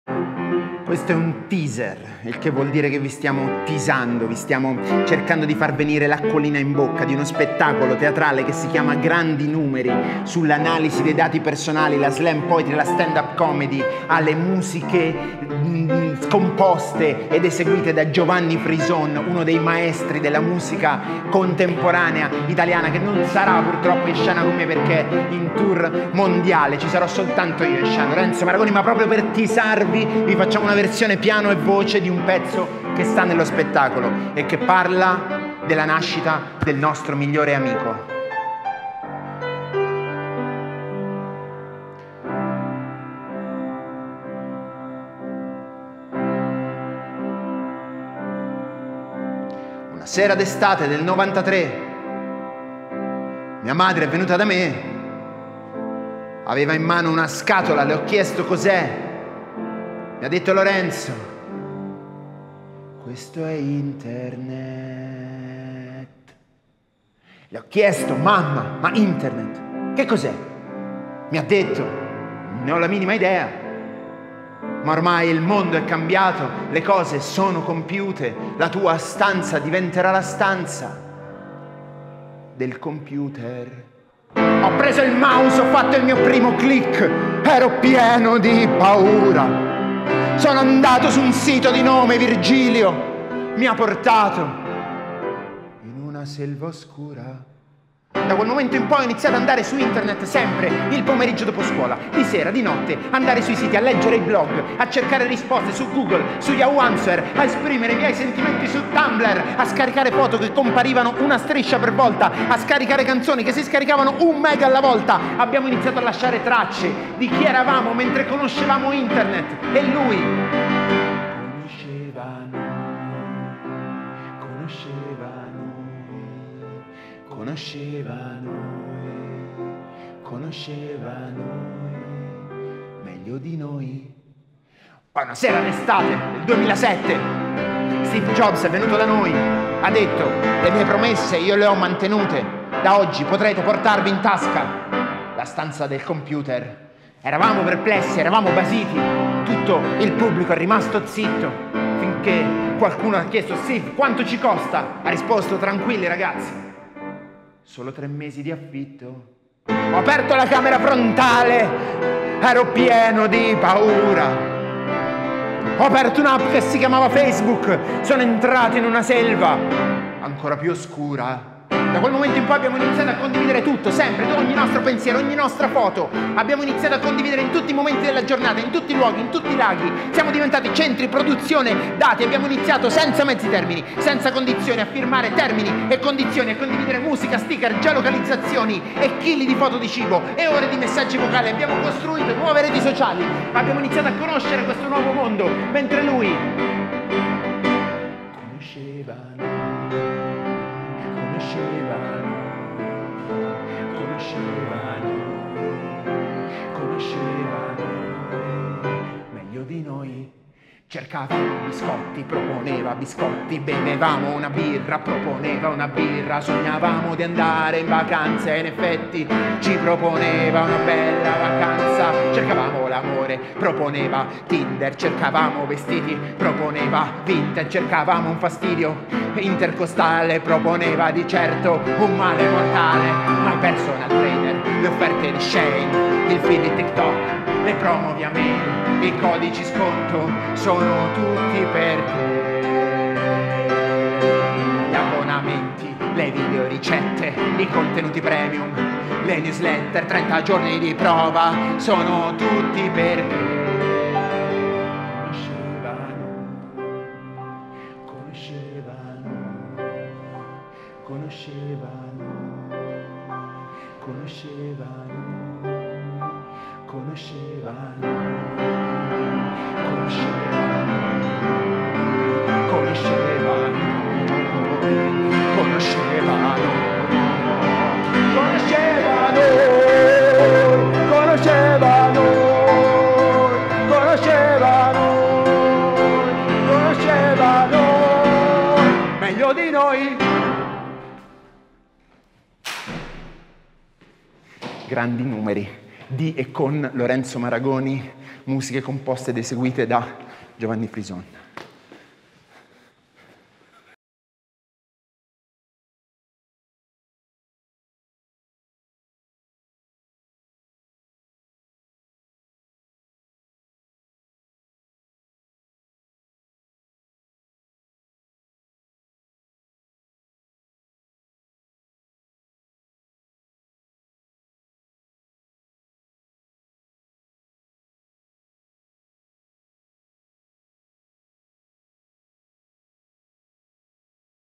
Teaser - Grandi Numeri
GRANDI NUMERI
Uno spettacolo di slam poetry, stand-up comedy e analisi dei dati personali

di e con Lorenzo Maragoni
musiche originali e sound design Giovanni Frison
assistente alla drammaturgia e alla regia Lucia Raffaella Mariani
luci Massimo Galardini

coordinamento tecnico dell’allestimento Marco Serafino Cecchi
assistente all’allestimento Giulia Giardi
organizzatore di compagnia Daniele Filosi
cura della produzione Francesca Bettalli e Camilla Borraccino
ufficio stampa Cristina Roncucci
comunicazione Francesco Marini
foto e video documentazione Davide Santinello

produzione Teatro Metastasio di Prato/TrentoSpettacoli
con il sostegno di Fondazione Caritro/Provincia Autonoma di Trento